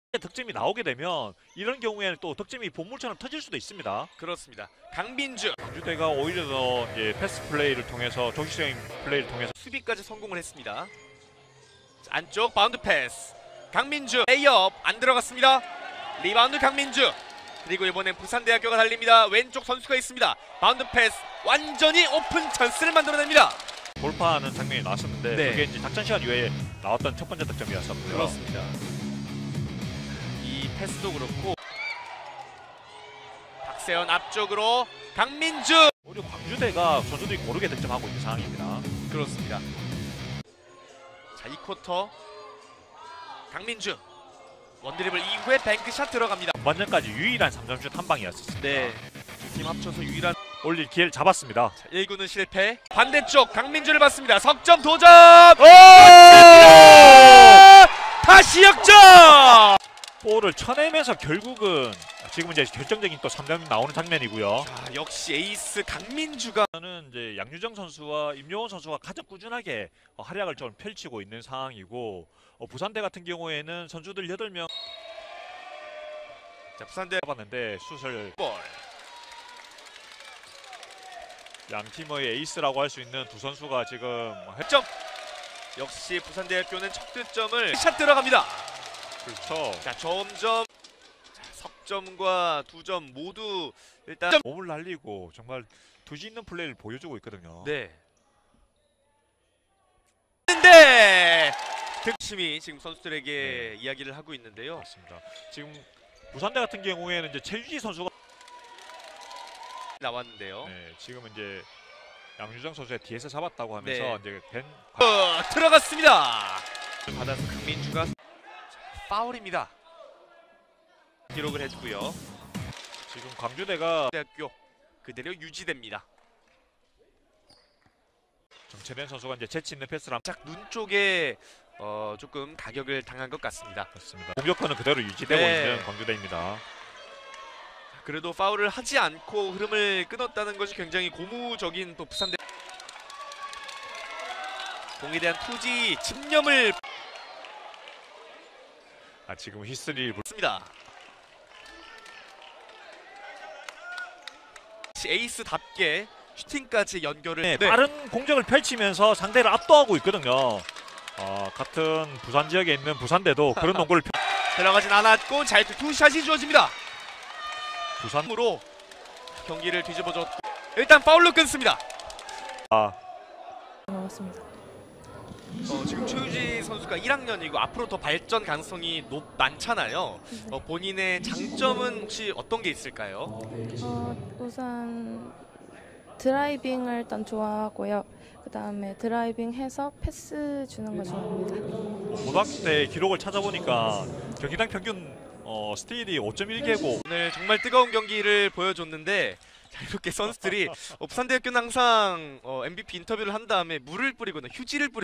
240409 대학농구 부산대 vs 광주대 경기 강민주 선수 cut
52:50 부산대 승

✨️14득 10리바 더블더블
01:06 까지 플레이

푸대 전승 가자❤️‍🔥